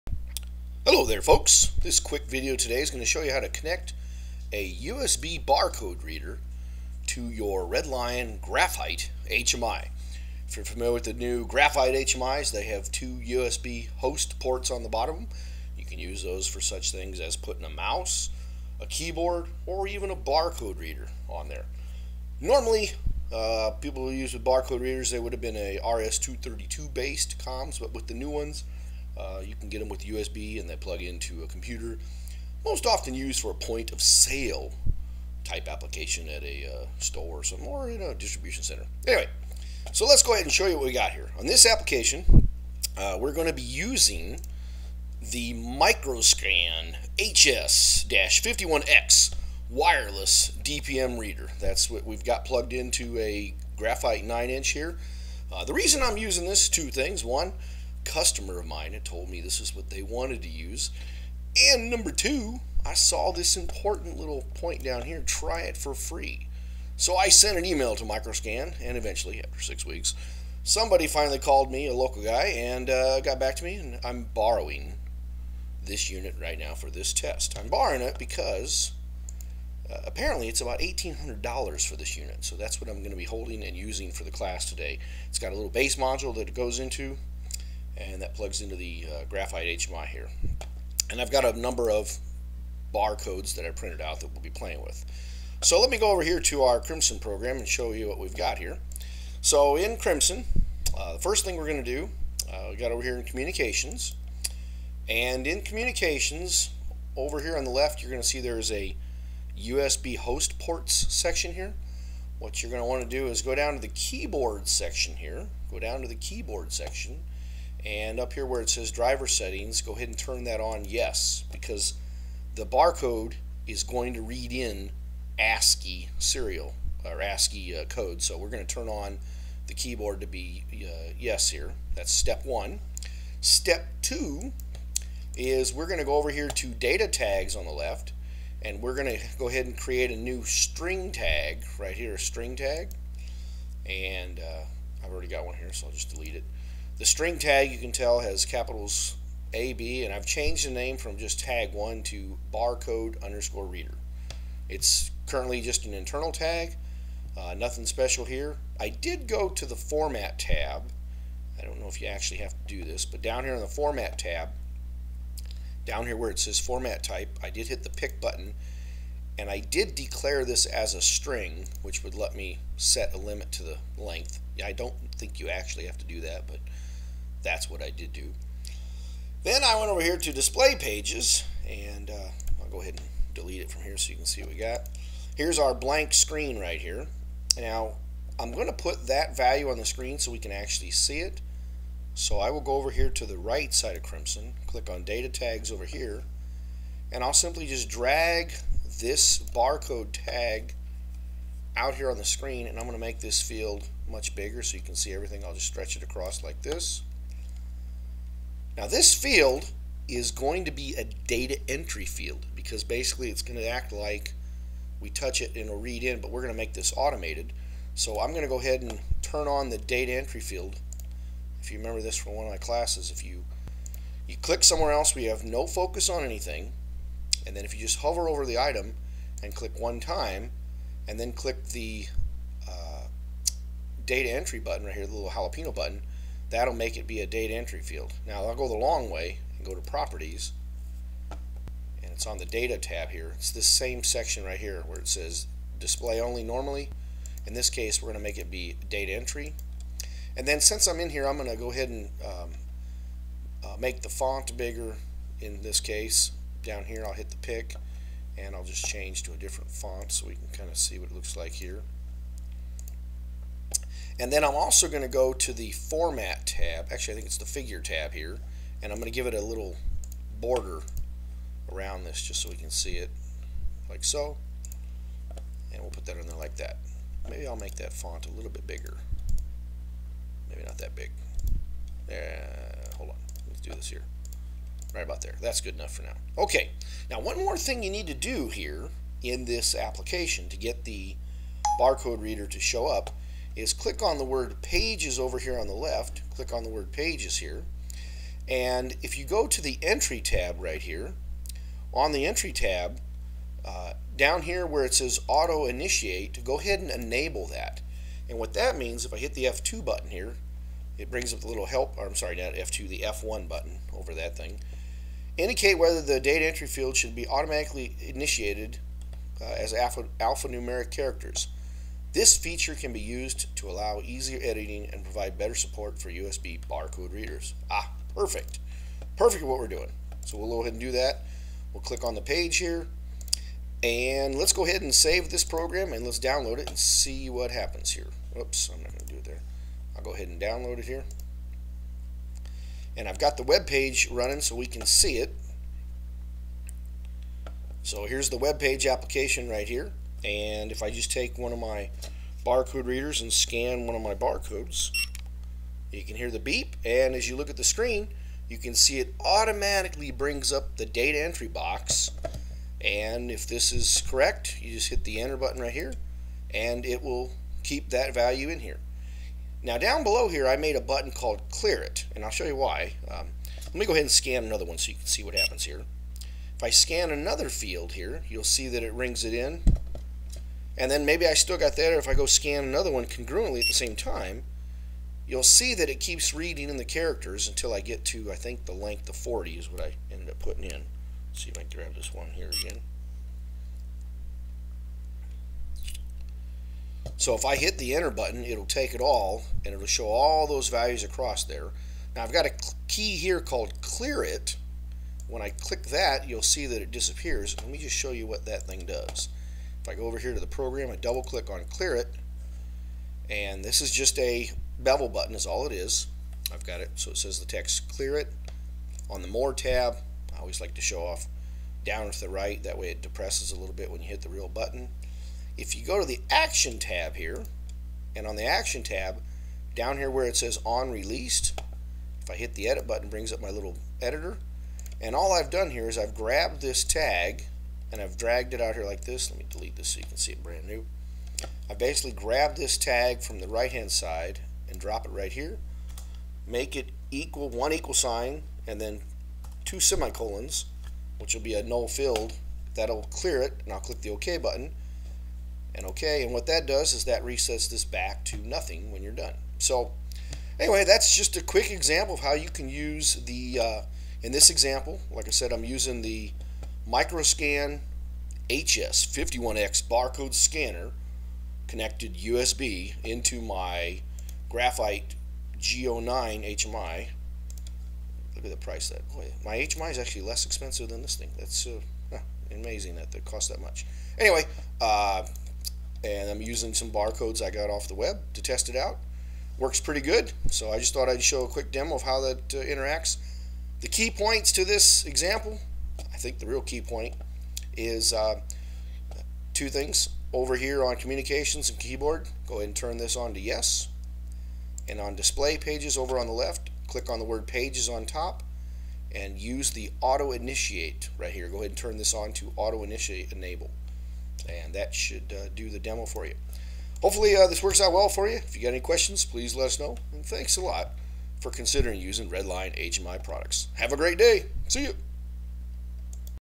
RedlIon Graphite HMI & USB BarCode Reader
Short video showing how the connect a MicroScan HS-51X USB BarCode reader to a RedLion Graphite HMI.